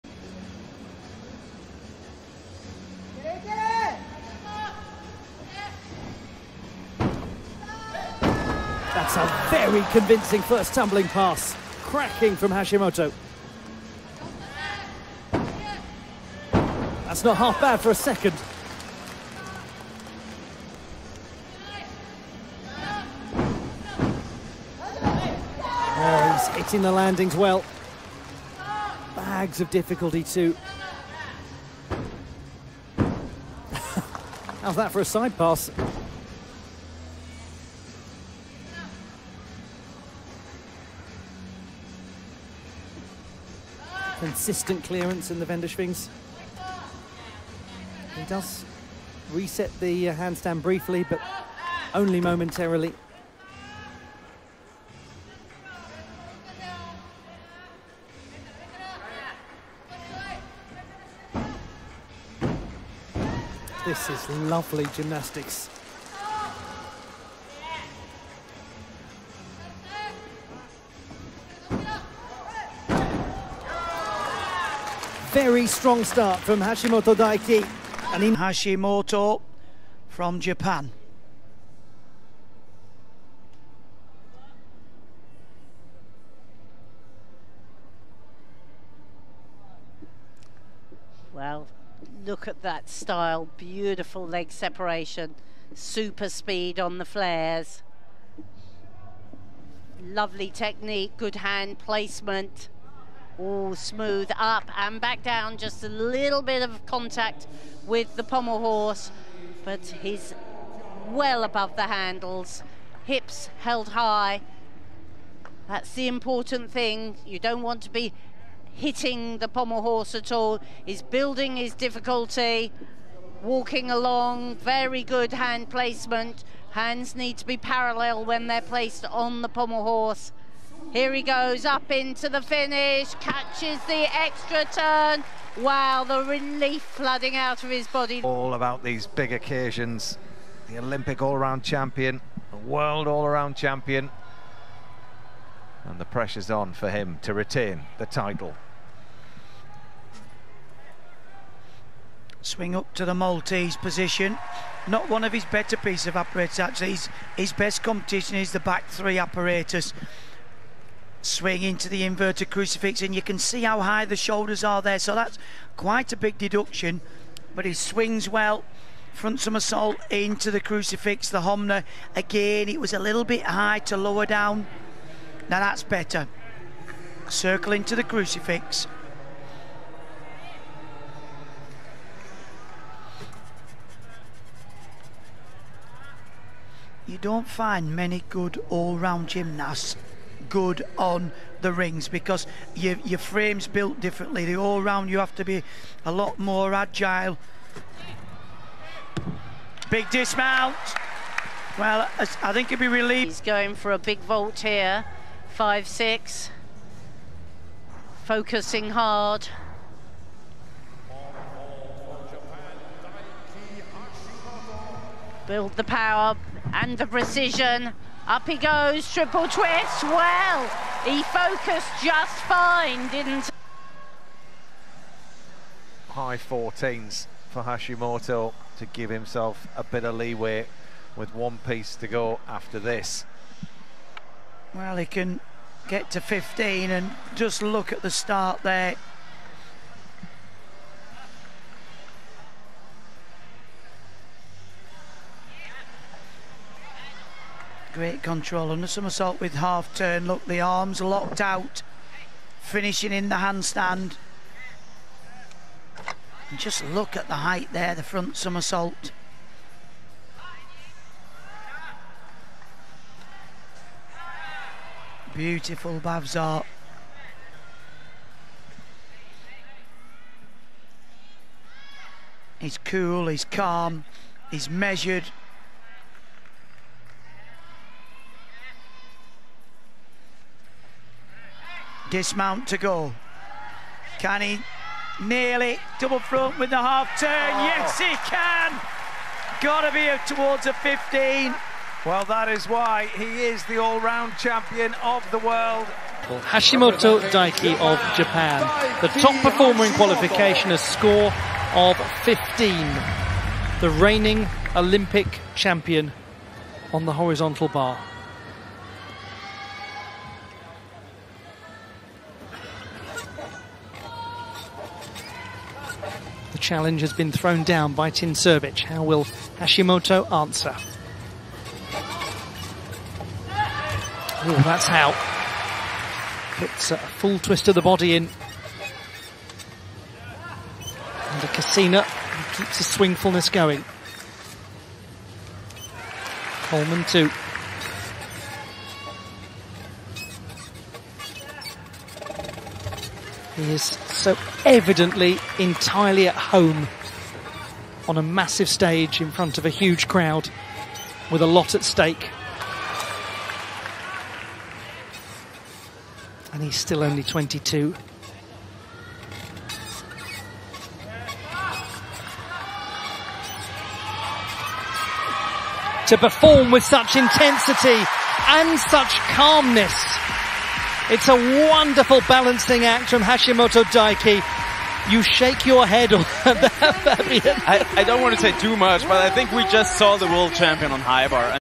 Best of Daiki Hashimoto
0:00 Floor
1:18 Pommel Horse
2:18 Rings
3:23 Vault
3:43 Parallel Bars
4:56 Horizontal Bar